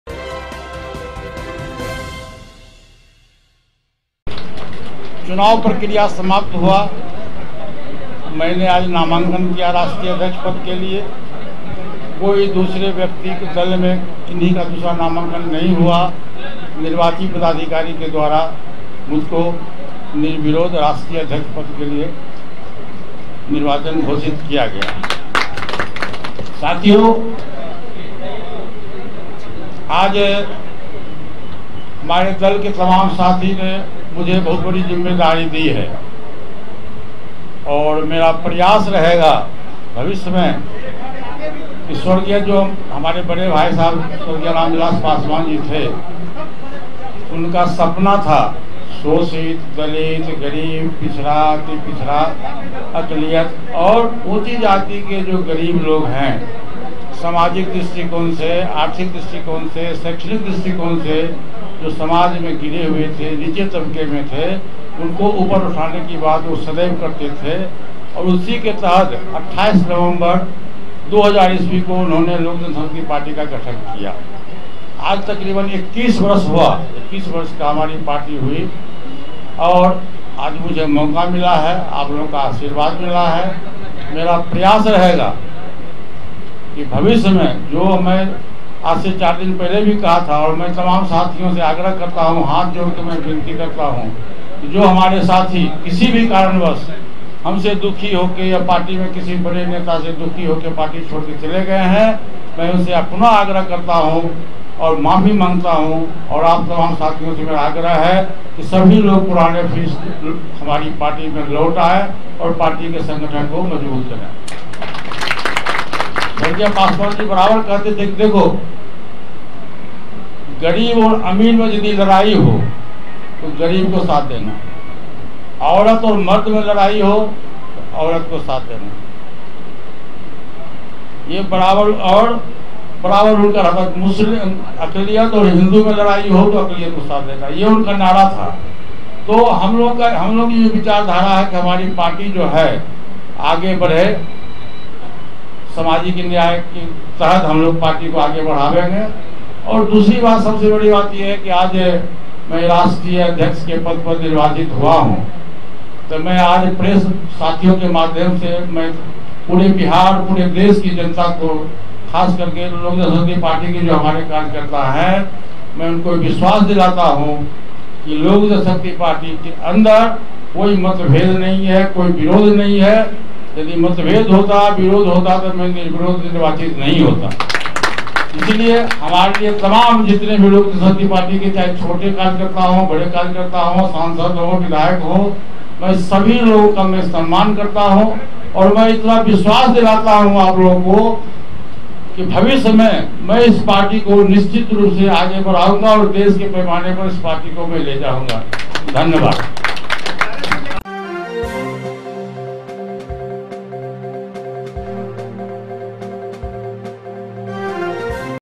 पशुपति पारस ने किया बड़ा ऐलान LJP से जितने नेताओं को निकाला गया सब को पार्टी में वापस आने का न्योता
Pashupati Paras ने किया बड़ा ऐलान LJP  से जितने नेताओं को निकाला गया सब को पार्टी में वापस आने का न्योता